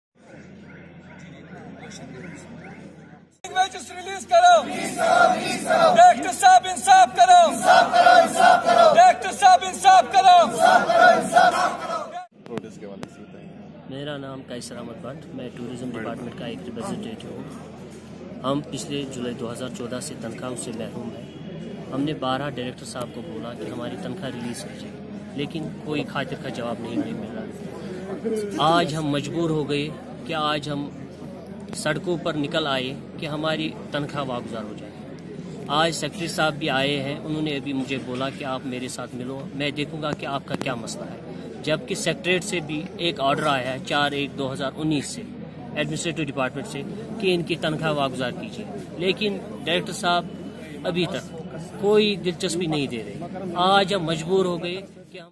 The Casual Labours Union of the Kashmir Tourism Department’s Tourist Reception Centre held a protest
The Casual Labours Union of the Kashmir Tourism Department’s Tourist Reception Centre (TRC) held a protest demonstration  in Srinagar on Friday , demanding the release of their long-pending wages.

Scores of casual labours assembled here in under the premises of Tourist Reception Centre Srinagar, chanting slogans in favour of their demands.

The protesters said that “our wages are pending from July 2014 till date, despite our continued service in the department”.